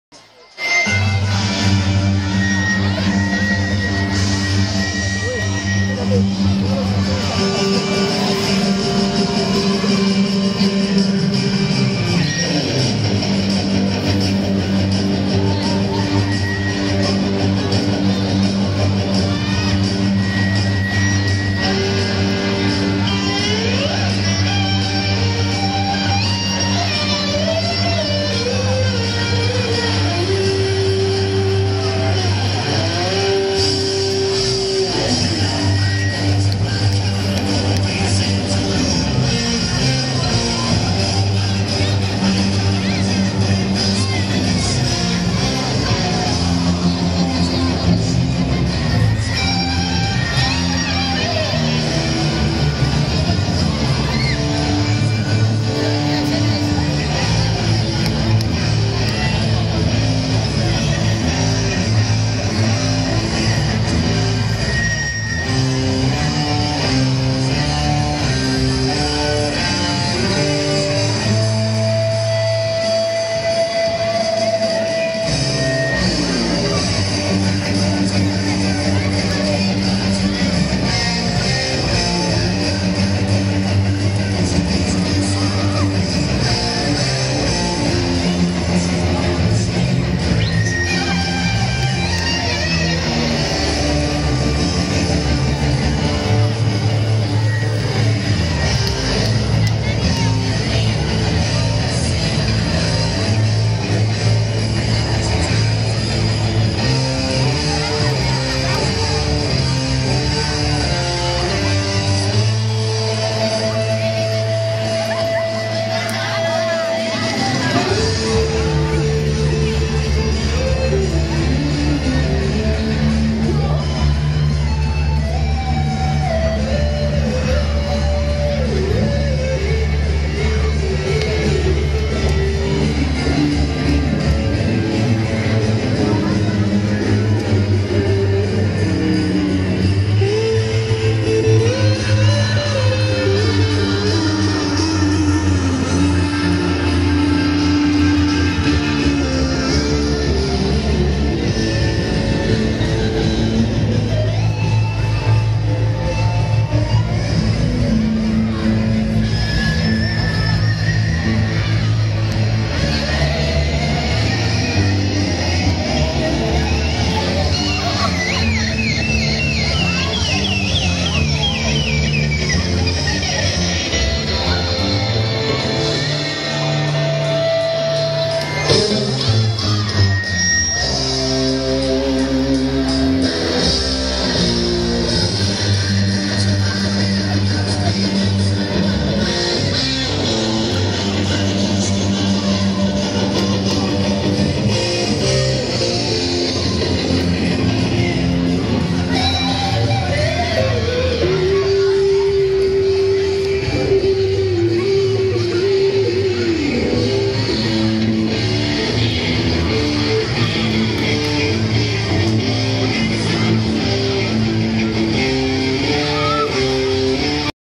1 campo valdes rock ok
kevin Mauricio, luciéndoos como siempre,con sus compañeros.
futuro músico de la ciudad y estudia en la i.e campo valdes y asiste al proyecto wayra dirigido por ángel esteban arredondo